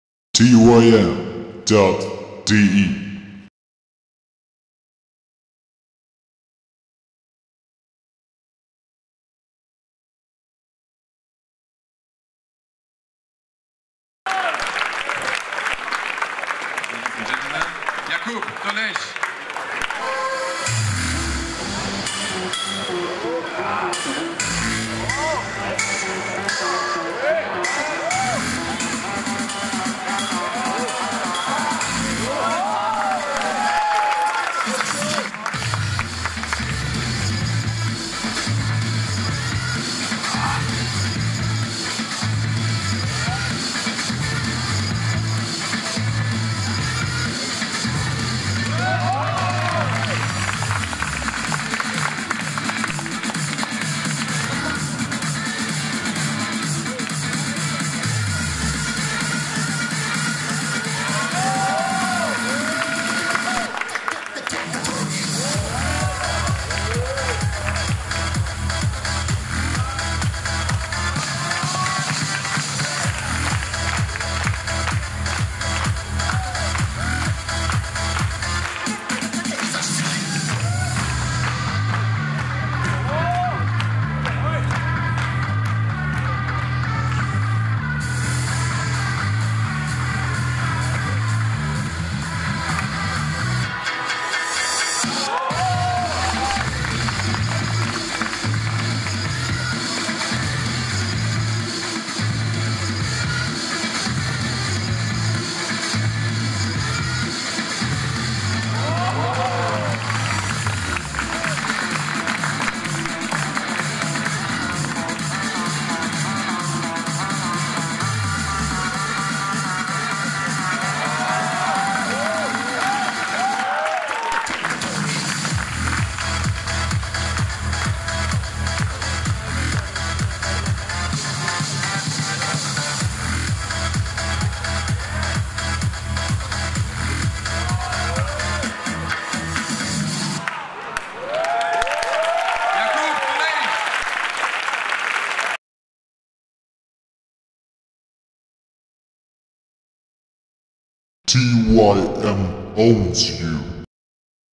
Deutsche Yo-Yo Meisterschaft 2022 2. Place 1A Open Jakub Dolejš
DM2022 German YoYo Nationals in Leipzig 1A Open International Guest Division
Wanted to do only a two minutes freestyle. - European Champion EYYC 2022